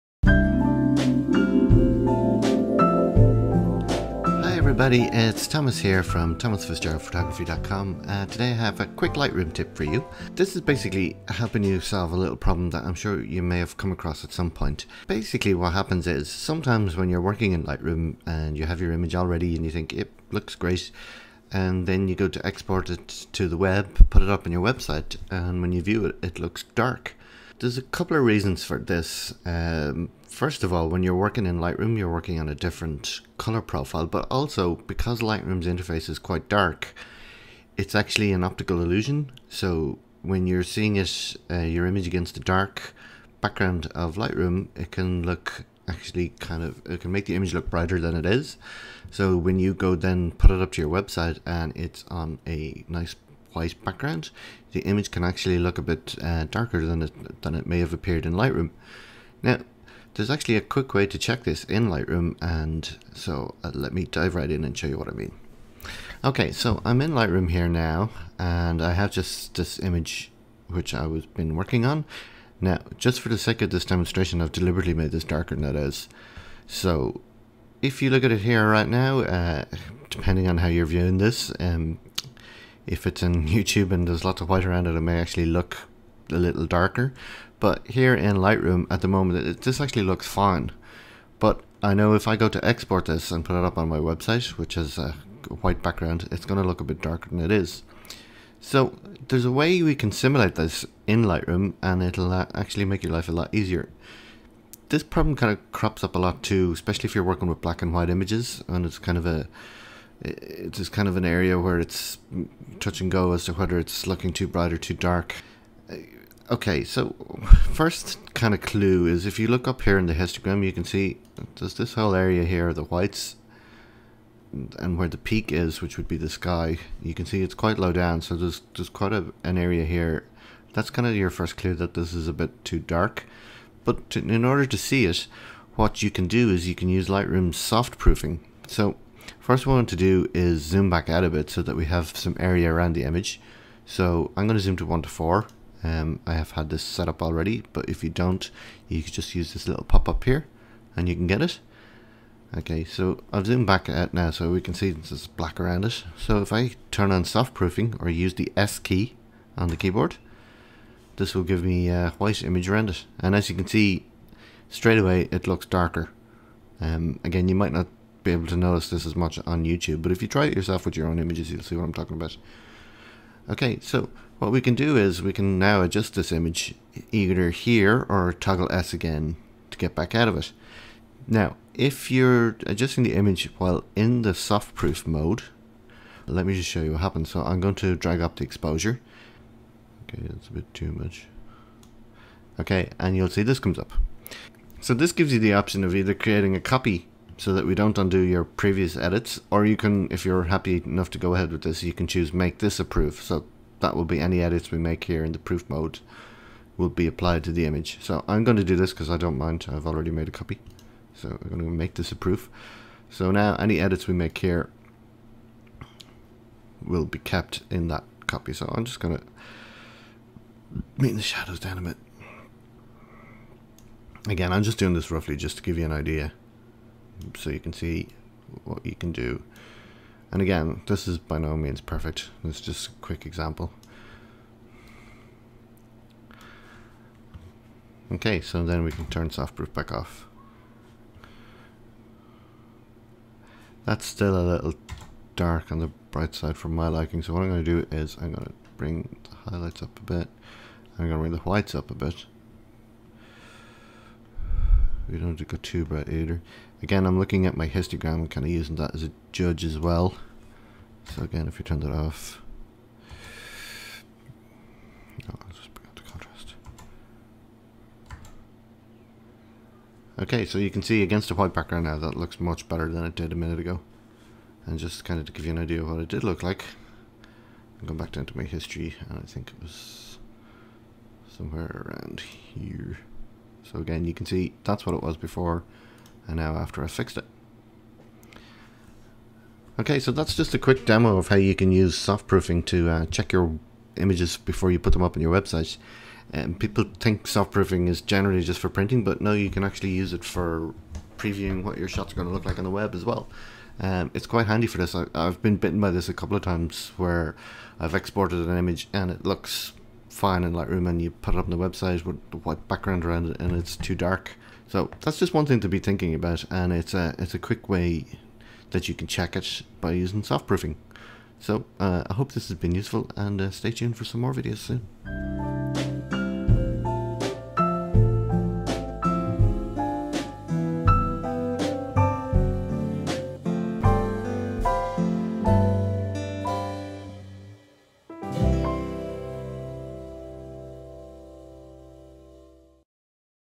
Using Soft Proofing in Lightroom to make sure your Images aren't too dark when you export to the web
A Quick Lightroom Screencast showing you how you can use Lightroom's Soft Proofing to make sure that your images won't be too dark when exported to the web